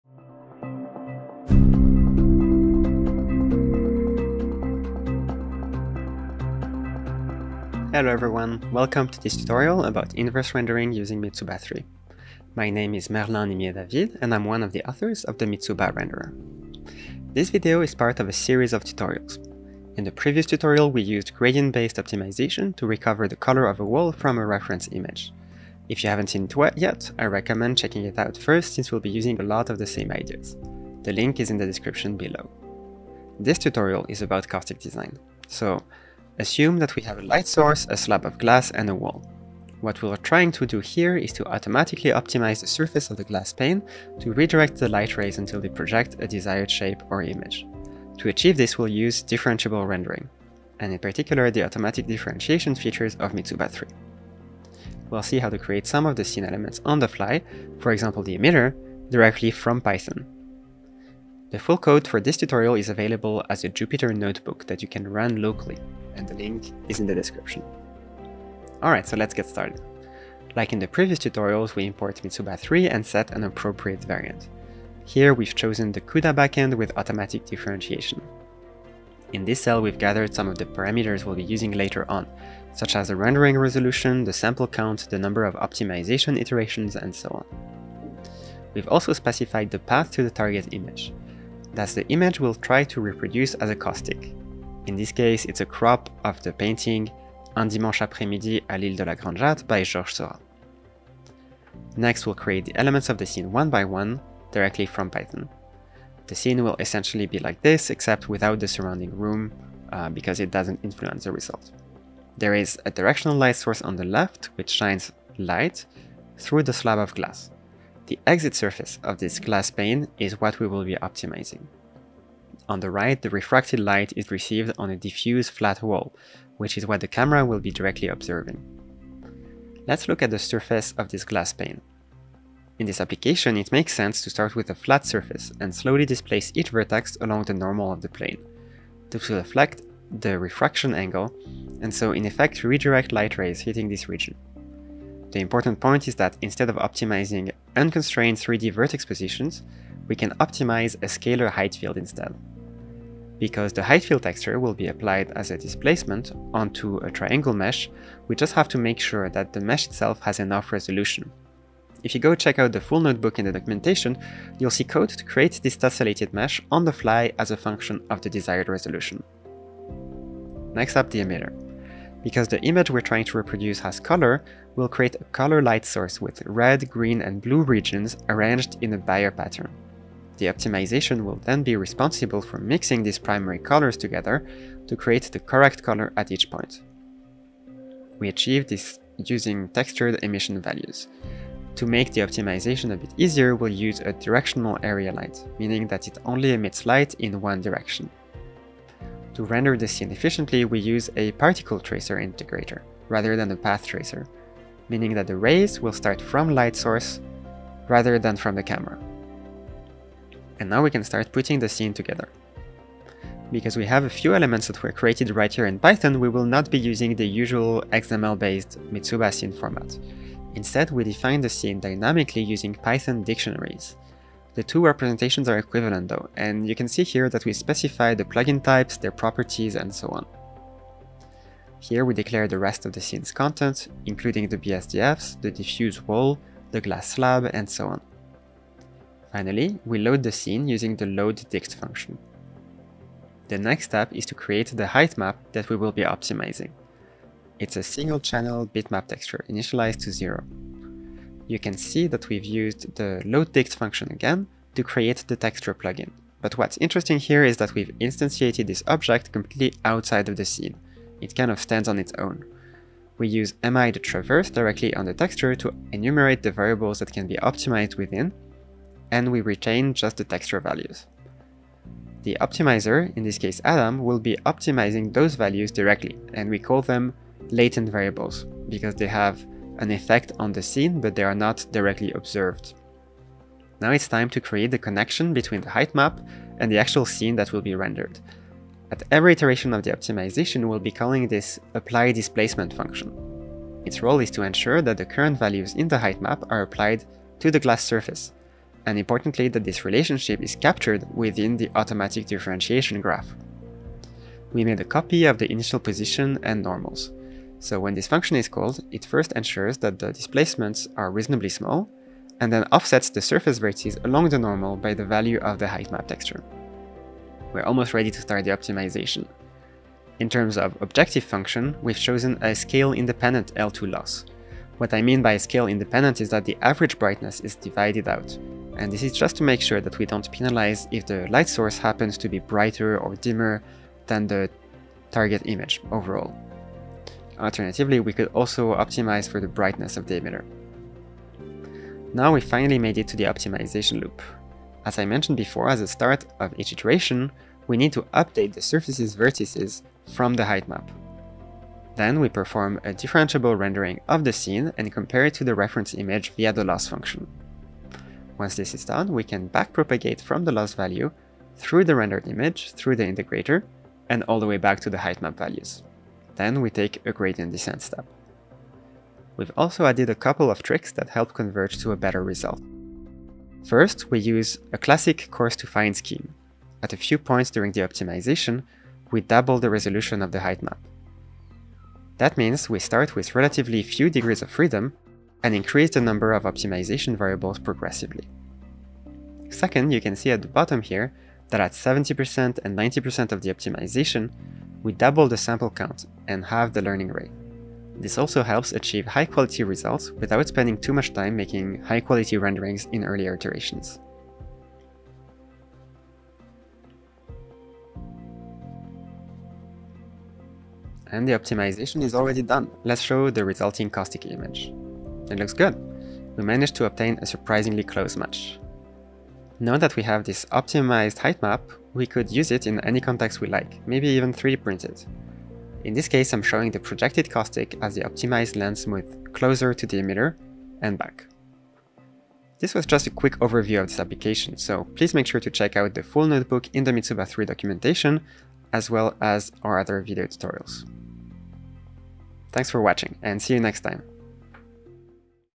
Mitsuba 3: Caustic Design (Tutorial #4)
This tutorial (part #4 of an ongoing series) by Merlin Nimier-David explains how to use Mitsuba 3 for "caustic design", i.e. to optimize the surface of a slab of glass in order to cast a desired caustic pattern.
- The full code for this tutorial is available as a Jupyter notebook